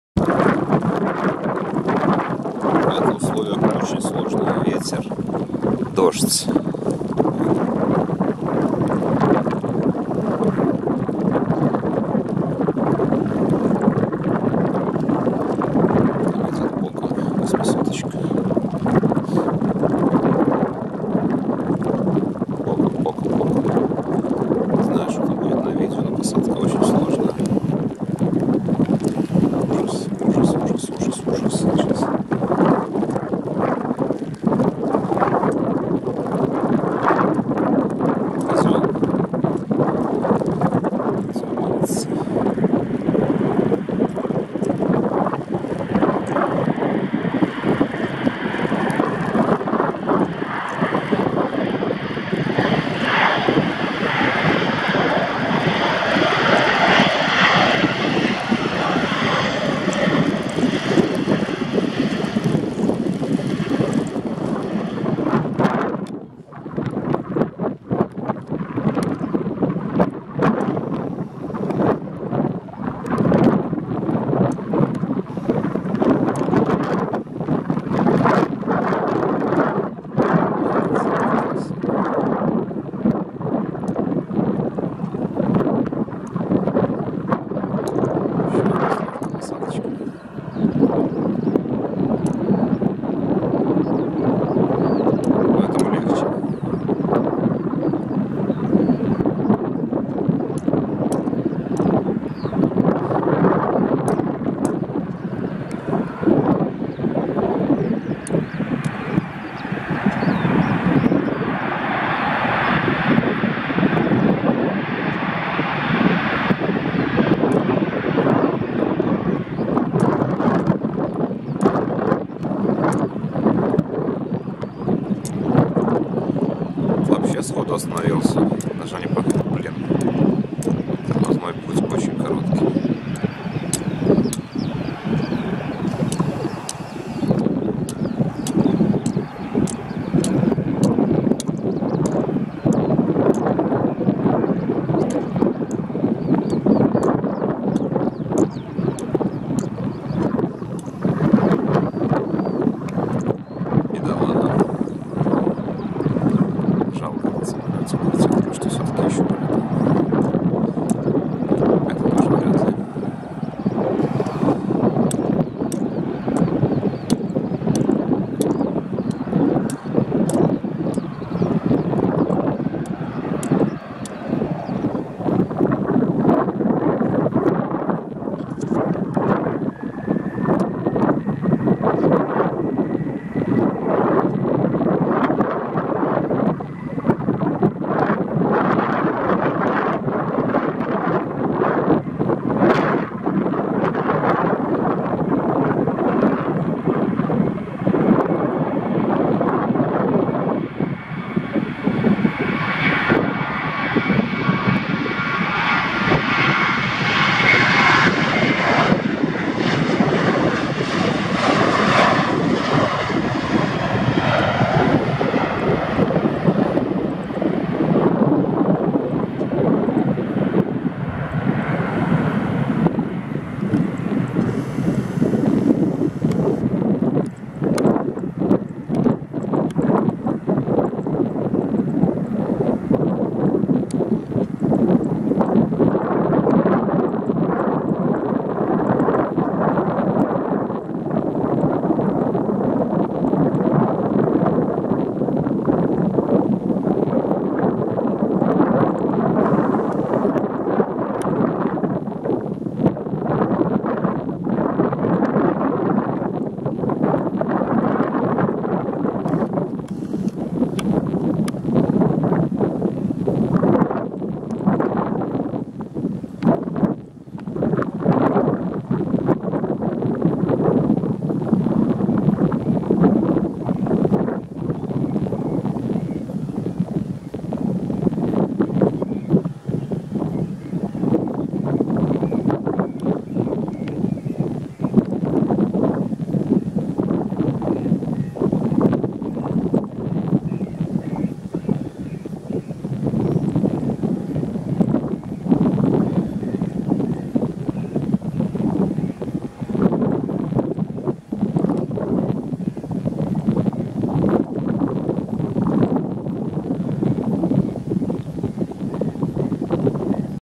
02.07.2018 Гомель (UMGG) Посадки-взлеты "Belavia"
2 посадки Boeing 737-800 Белавии в старой и новой ливрее. Можете сравнить разницу и почувствовать красоту новой. Обратите внимание на силу реверса в первом и во втором случае. Первый укатился на всю полосу, второй-остановка на половине и быстрый заезд к гейтам. Ну и взлет 800 с уходом и разворотом)